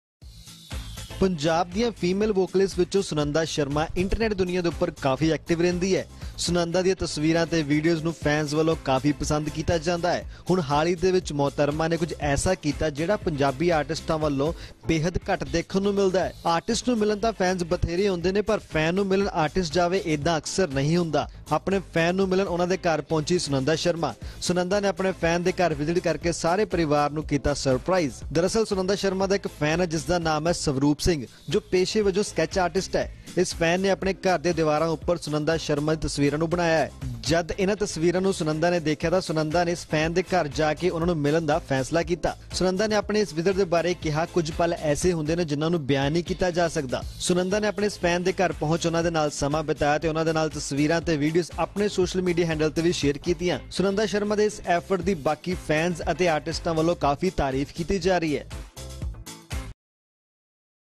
Sunanda Sharma Fan Moment | Fan Painted wall of his House with Sunanda Sharmas' Picture | Abp sanjha
ਸੁਨੰਦਾ ਸ਼ਰਮਾ ਪਹੁੰਚੀ ਆਪਣੇ ਫੈਨ ਨੂੰ ਮਿਲਣ ਉਸ ਦੇ ਘਰ
ਫੈਨ ਨੇ ਘਰ ਦੀਆ ਦੀਵਾਰਾਂ 'ਤੇ ਬਣਾਈਆਂ ਸੁਨੰਦਾ ਦੀਆਂ ਤਸਵੀਰਾਂ
ਸੁਨੰਦਾ ਸ਼ਰਮਾ ਨੇ ਫੈਨ ਦੇ ਘਰ ਵਿਜ਼ਿਟ ਕਰ ਦਿੱਤਾ ਸਰਪ੍ਰਾਈਜ਼
ਸੁਨੰਦਾ ਦੀਆਂ ਇਹ ਤਸਵੀਰਾਂ ਤੇ ਵੀਡਿਓਜ਼ ਇੰਟਰਨੈਟ 'ਤੇ ਵਾਇਰਲ